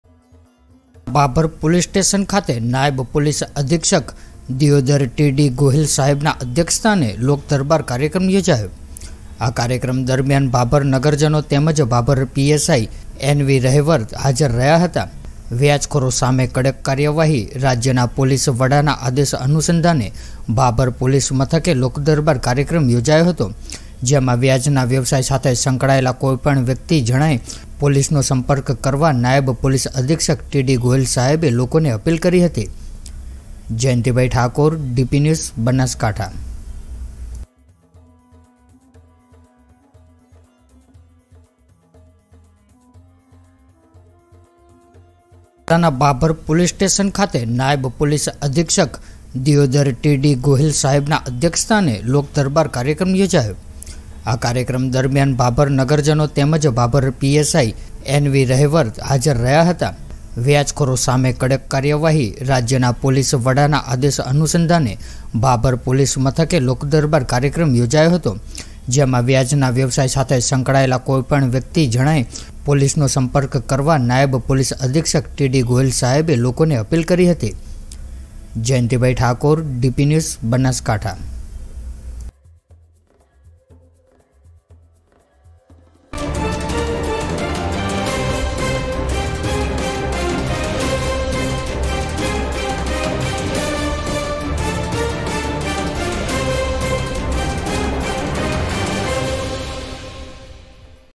Bhabhar Police Station | બનાસકાંઠા: ભાભર પોલીસ સ્ટેશન ખાતે લોકદરબાર યોજાયો| Banaskantha News| Dpnews
બનાસકાંઠા: ભાભર પોલીસ સ્ટેશન ખાતે લોકદરબાર યોજાયો
About Video: બનાસકાંઠા:ભાભર પોલીસ સ્ટેશન ખાતે નાયબ પોલીસ અધિક્ષક દિયોદર ટી ડી ગોહિલ સાહેબ ના અધ્યક્ષસ્થાને લોકદરબાર યોજાયો.

આ કાર્યક્રમ દરમ્યાન ભાભર નગરજનો તેમજ ભાભર પીએસઆઈ એન વી રહેવર હાજર રહ્યા હતા..

વ્યાજખોરો સામે કડક કાર્યવાહી ના રાજ્ય પોલીસ ના વડાના આદેશ અનુસંધાને ભાભર પોલીસ મથકે લોકદરબાર યોજાયો હતો..

જેમાં વ્યાજ ના વ્યવસાય સાથે સંળાયેલ કોઈ પણ વ્યક્તિ જણાય તો પોલીસ નો કોન્ટેક્ટ કરવા નાયબ પોલીસ અધિક્ષક ટી ડી ગોહિલ સાહેબ લોકો ને અપીલ કરી.

બાઈટ DYSP ટી ડી ગોહિલ

રિપોર્ટર...જયંતિ ઠાકોરે ભાભર બનાસકાંઠા